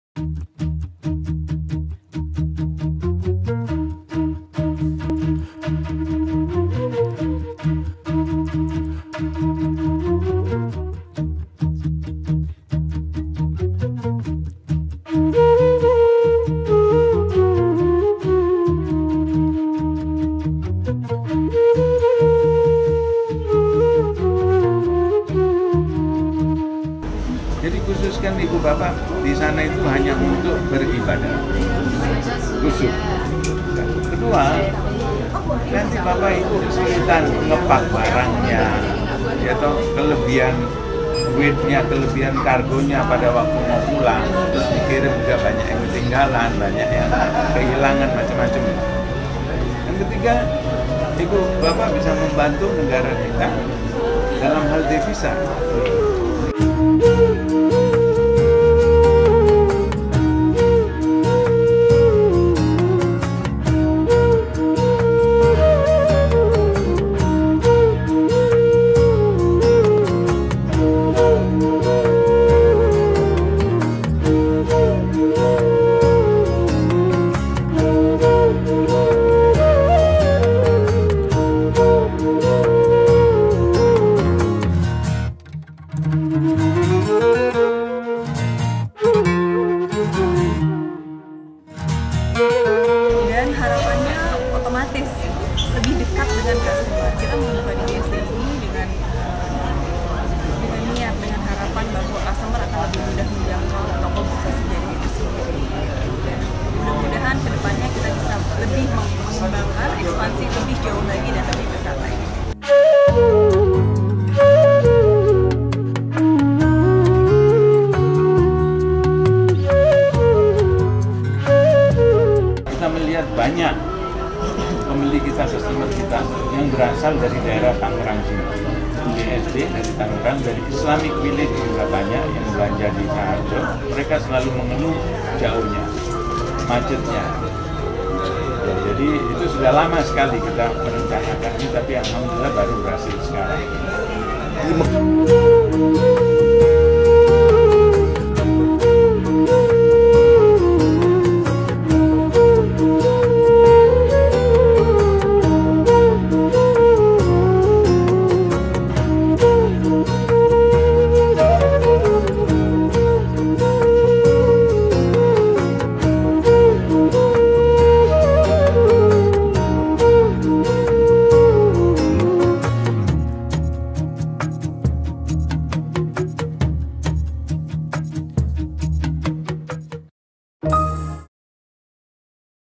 Bursa Sajadah Cabang BSD Tangerang Selatan
"Bursa Sajadah BSD 1st Anniversary"

Tepat tanggal 31 Maret 2018, Bursa Sajadah cabang BSD telah melalui tahun pertama dengan pencapaian yang sangat baik. Ditahun pertama, Bursa Sajadah BSD telah melayani ribuan customer dalam memenuhi kebutuhan akan perlengkapan dan oleh-oleh haji/umroh.

Keberadaan cabang terbaru yang bertempat di BSD Tangerang Selatan ini semakin mengokohkan Bursa Sajadah sebagai toko pusat perlengkapan muslim TERBESAR dan TERLENGKAP di Indonesia.

Kini sahabat Bursa Sajadah yang berada di kota Tangerang bisa lebih mudah dalam mendapatkan berbagai kebutuhan perlengkapan ibadah dan oleh-oleh haji/umroh.